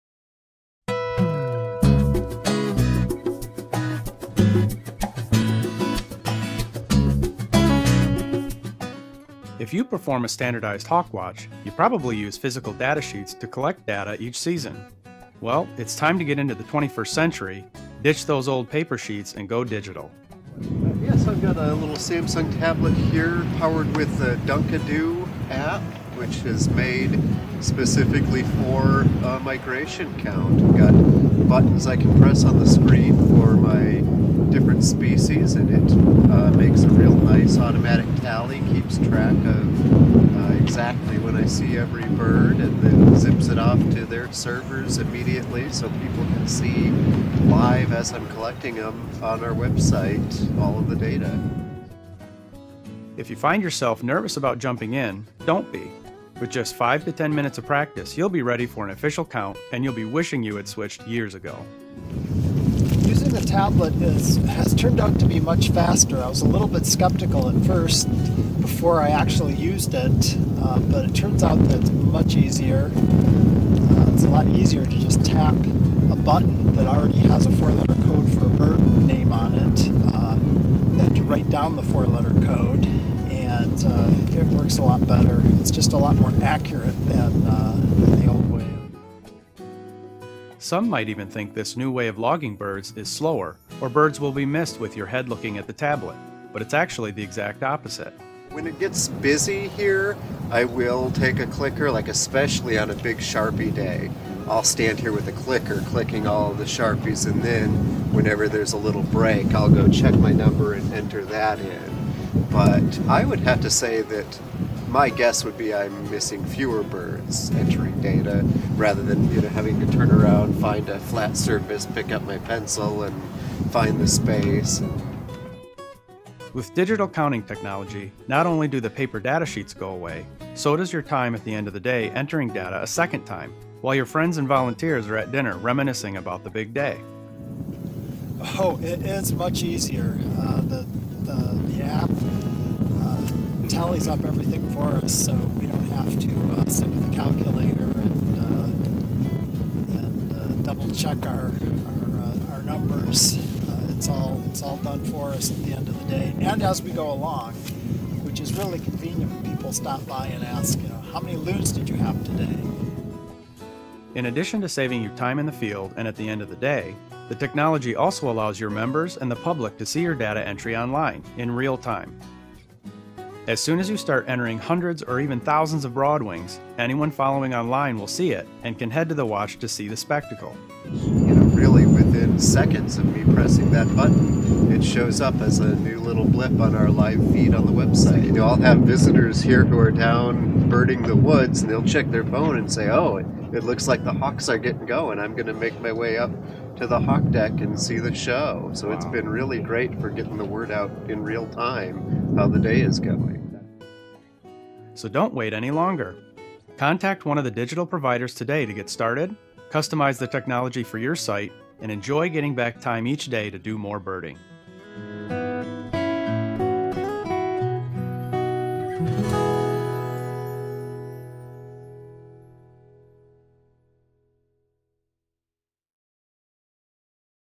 Intro to Hawk Watch Tablet Data Entry
An introduction to how digital technology is advancing the counting process at hawk migration sites across North America.  The way data is tallied, shared and uploaded is improving by leaps and bounds through the use of tools such as Dunkadoo and Trektellen.